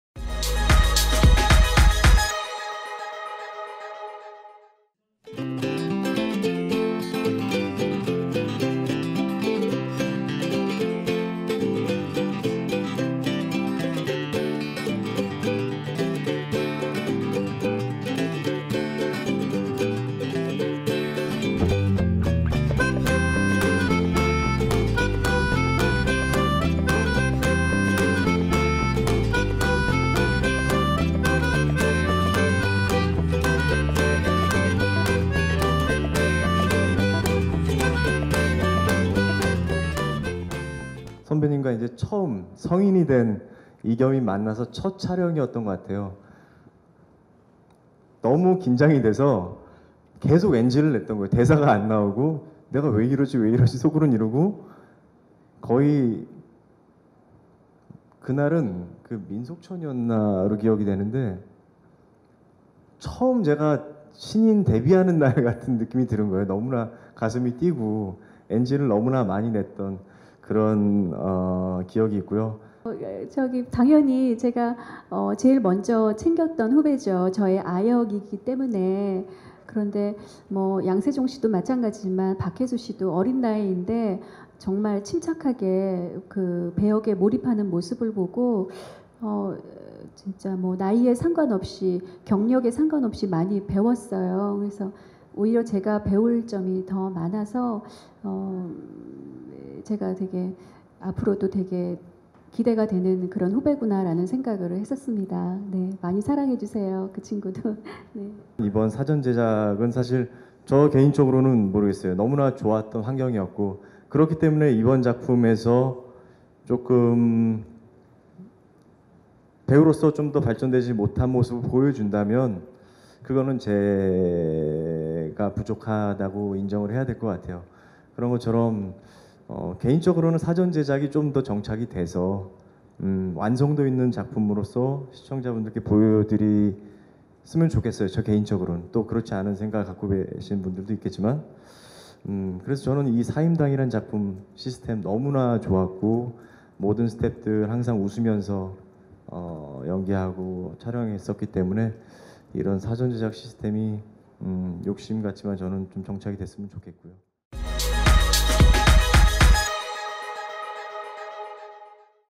[tvdaily] '사임당' ★송승헌★ "이영애와 첫 연기, 떨려서 NG 계속 냈다"
[티브이데일리 최현규 기자] SBS 새 수목드라마 '사임당' 제작발표회가 24일 오후 서울 중구 소공동 롯데호텔에서 열렸다.

이날 제작발표회에는 배우 이영애 송승헌 오윤아 양세종, 윤상호 감독, 박은령 작가가 참석했다.

'사임당'은 조선시대 사임당 신씨의 삶을 재해석해 그의 예술혼과 불멸의 사랑을 그린 드라마로 오는 26일 첫 방송된다.